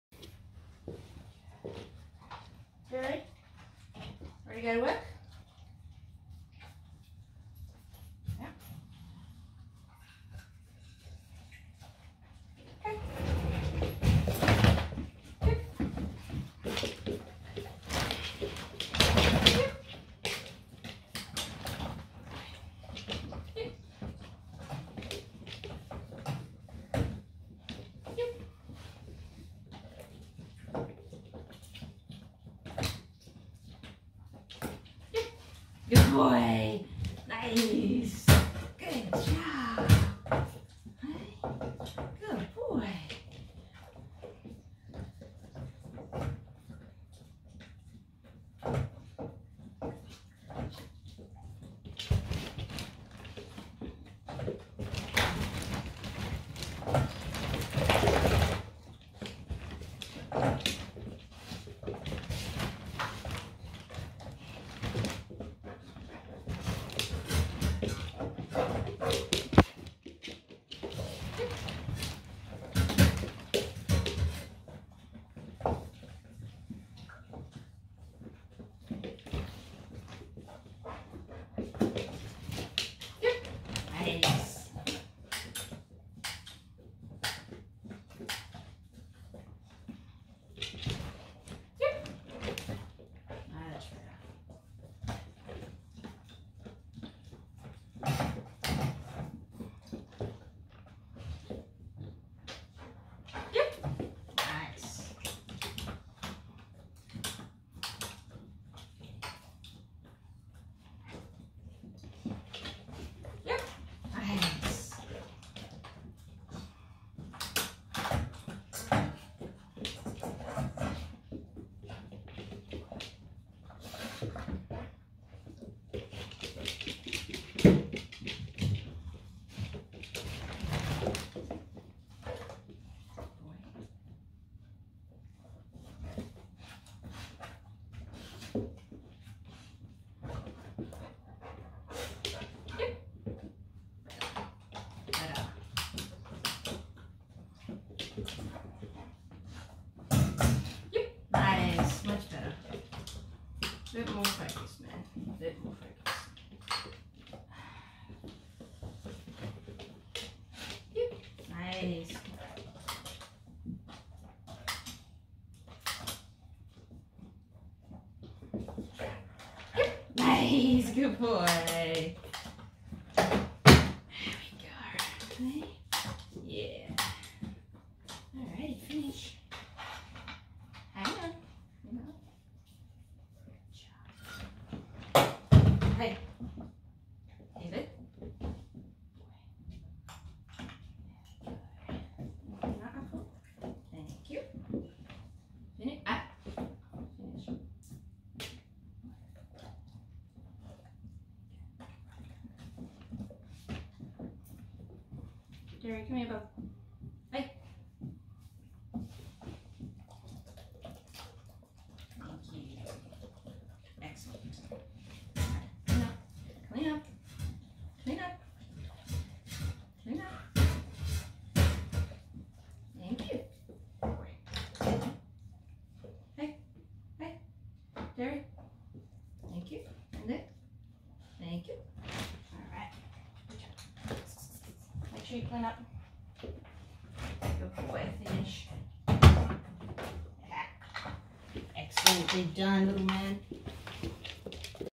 The Derry Project
🤔What is this video all about🧐: Dog demo of Derry learning how to switch the lightswitch ON/OFF via using Shaping (20220724)

Show PCC some support🤝 by buying or sharing one of my designs, and the $$ will be used to rehabilitate Rescue dogs from SAFE Inc

🤓🙃Follow PCC's journey on FB: @PerthCanineCraft🐾